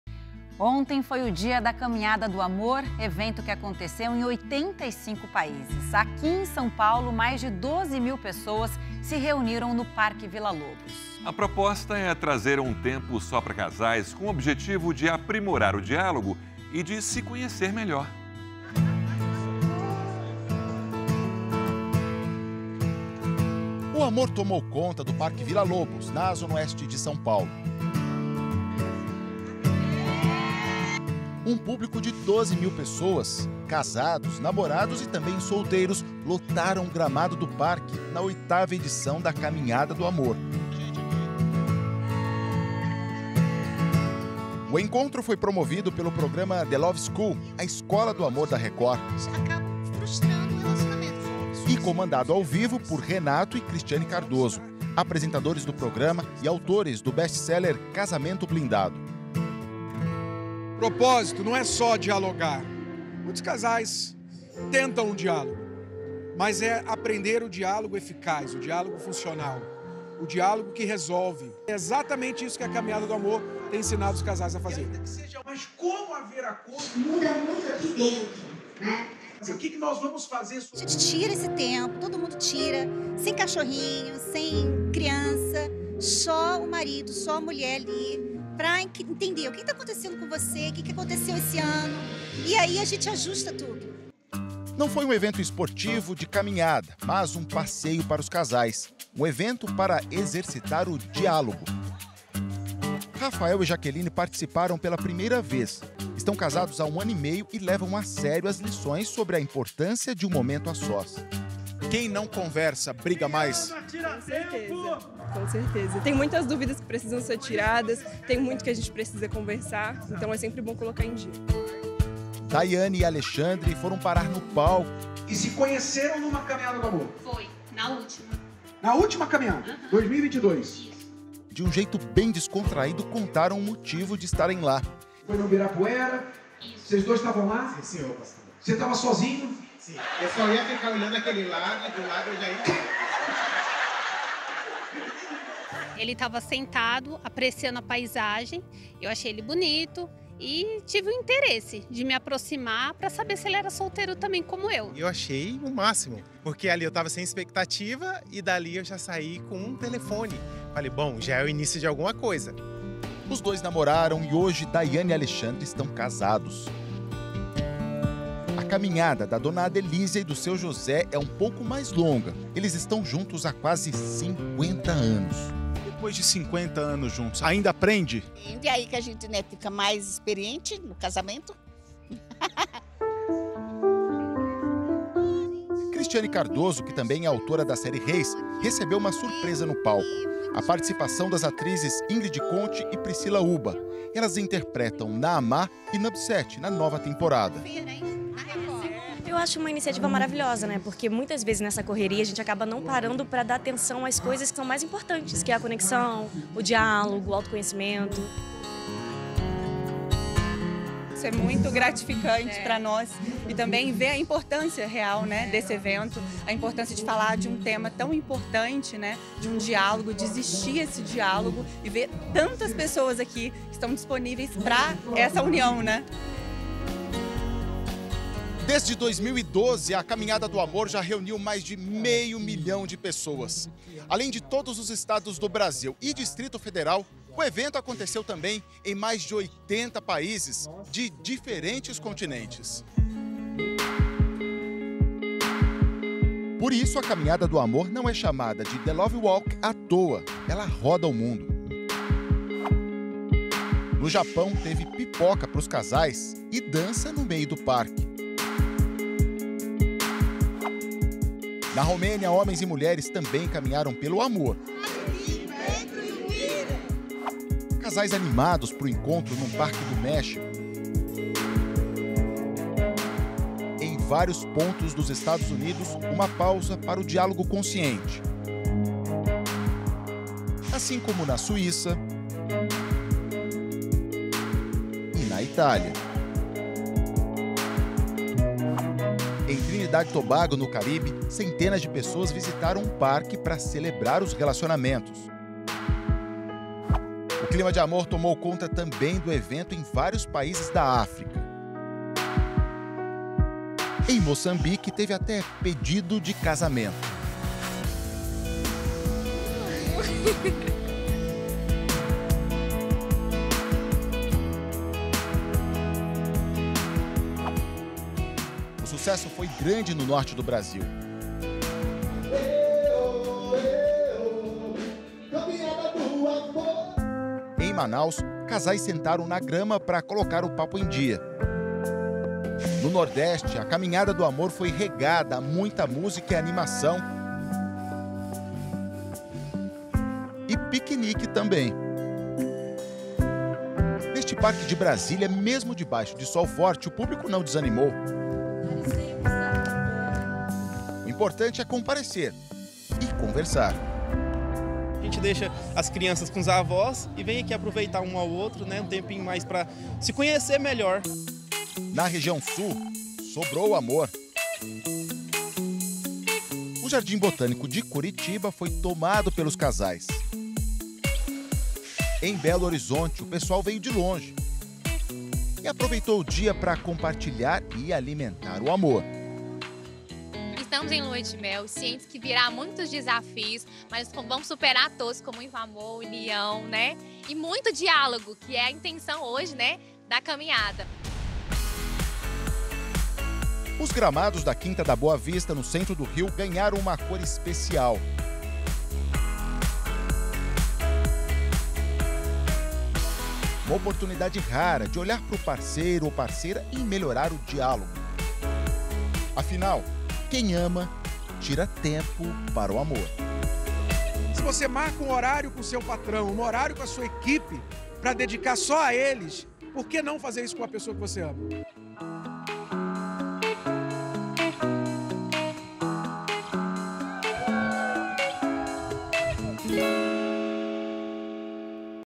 Milhares de casais em 85 países diferentes se encontram na Caminhada do Amor
O último sábado (21) marcou o dia da ‘Caminhada do Amor’, evento que aconteceu em 85 países. Só aqui em São Paulo, mais de 12 mil pessoas se reuniram no Parque Villa-Lobos. A proposta foi trazer um tempo de qualidade só para casais com o objetivo de aprimorar o diálogo no relacionamento.